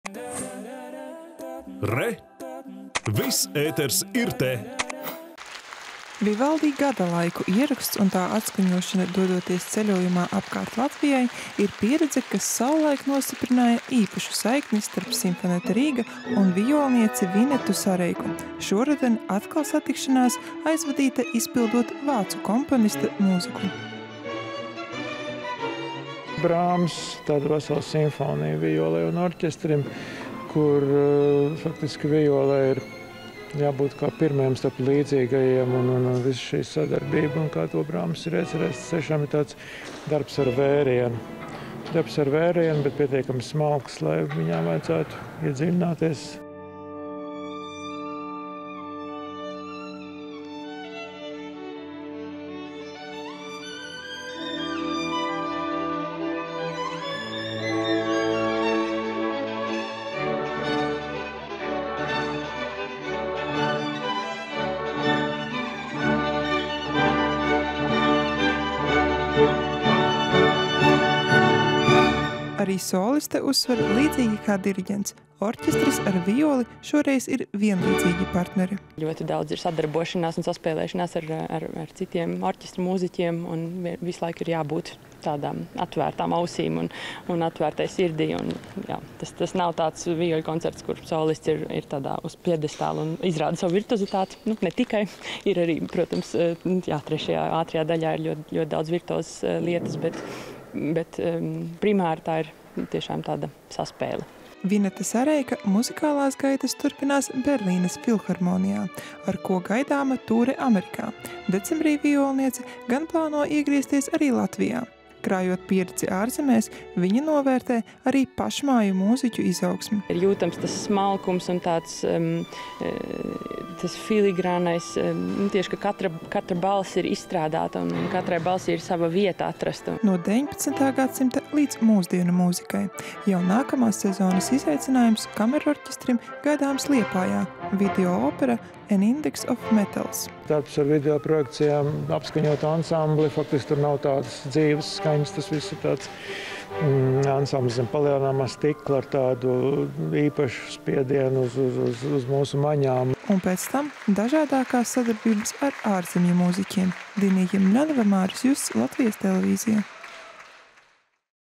“Sinfonietta Rīga” atklāj sezonu saspēlē ar Vinetu Sareiku
Ar diviem vakariem Roberta Šūmaņa un Johanesa Brāmsa skaņdarbu virpulī Lielajā Ģildē un koncertzālē “Gors” aizvadītajā nedēļas nogalē jauno sezonu sāka kamerorķestris “Sinfonietta Rīga”. Jaunās sezonas iesākumā orķestris atkal satikās ar vijolnieci Vinetu Sareiku. Kādi muzikāli izaicinājumi sekos turpmākajā sezonā, noskaidroja Dinija Jemeļjanova.